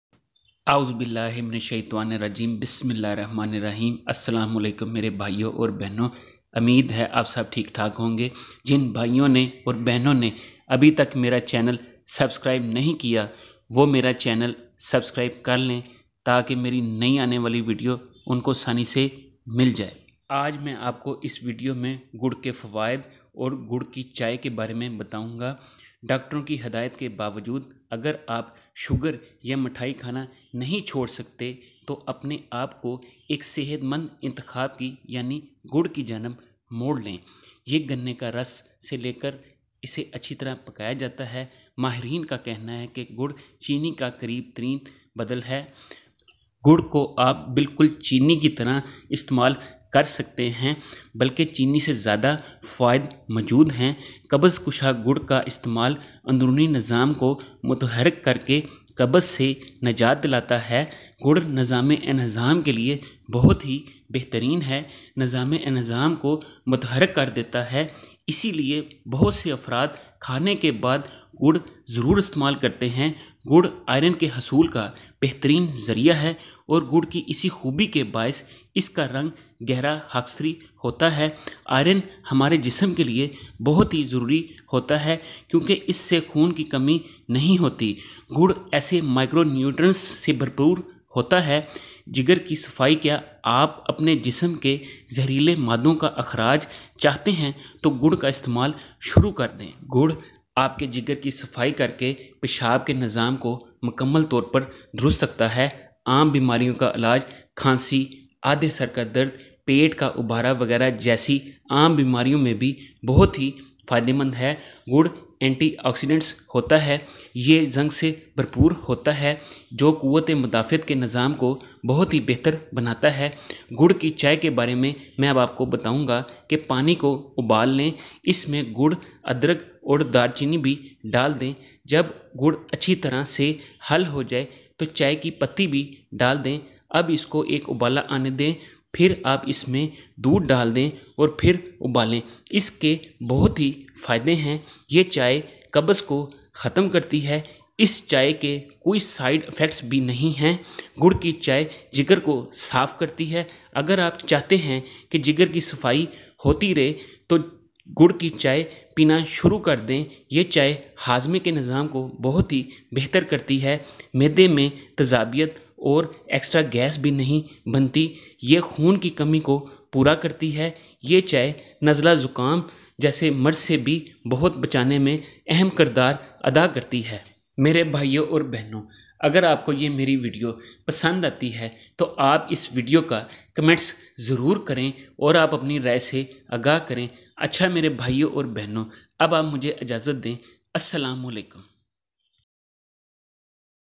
Eat Jaggery On Empty Stomach Then See What Happens Truth Was Hidden From Everyone, Urdu, Hindi
Yeah Allah pak kee taraf say bohat baree naymat Hai jaggery ke bohat fayde haan Iss video mein aap koo iss Kay fayday bataon gaaa